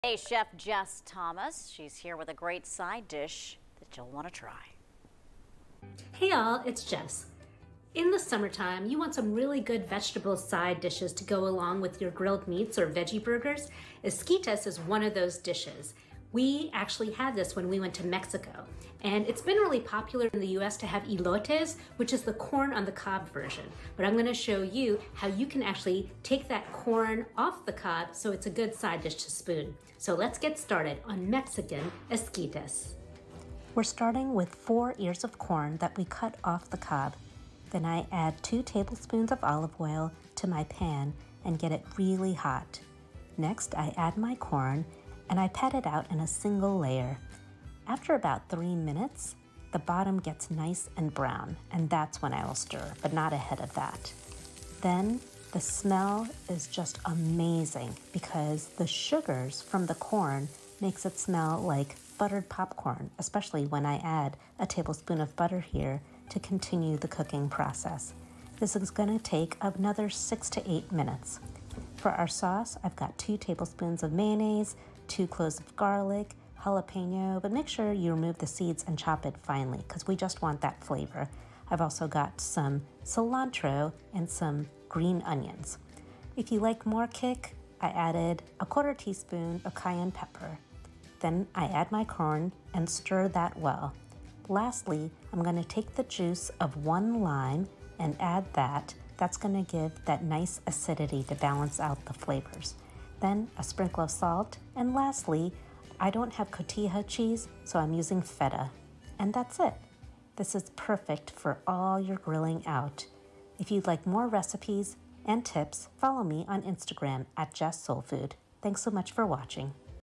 Esquites Mexican Corn Salad
Chef Jes Thomas makes a corn salad recipe. Follow her on Instagram @jessoulfood. June 22, 2021-4pm.